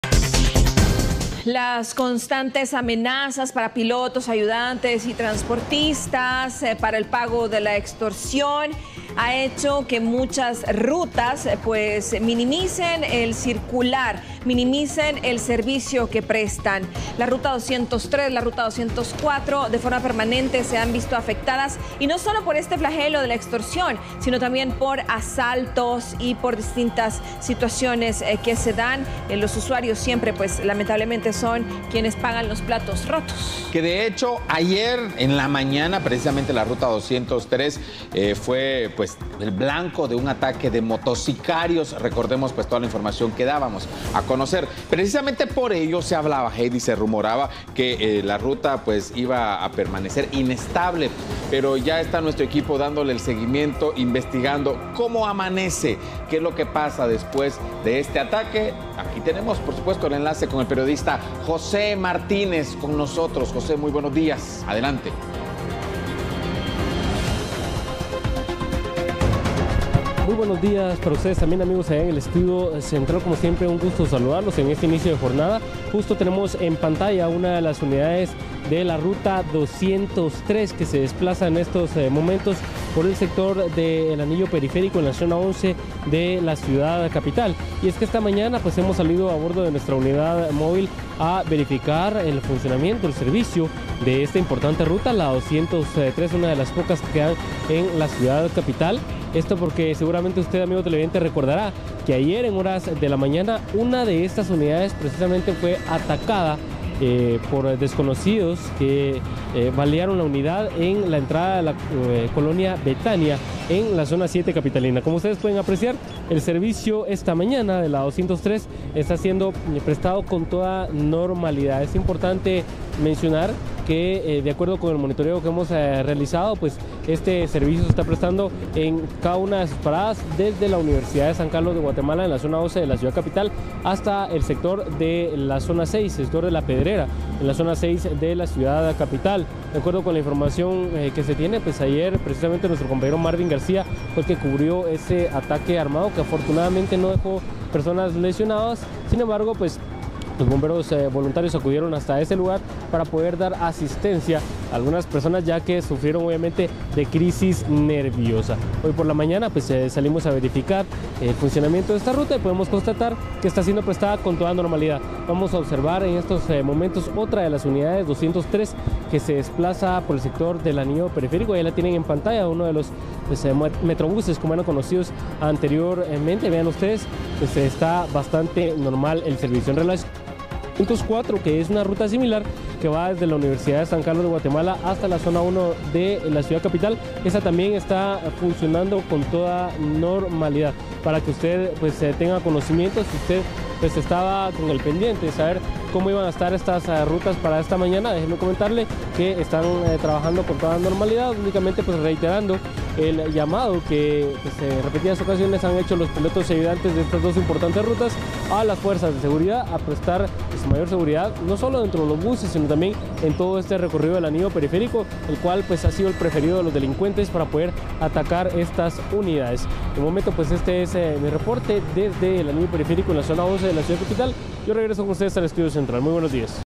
Normalidad en el servicio de la ruta 203
Pilotos son blanco de ataques
Los conductores prestan el servicio de transporte pese al ataque armado registrado en contra de uno de los trabajadores.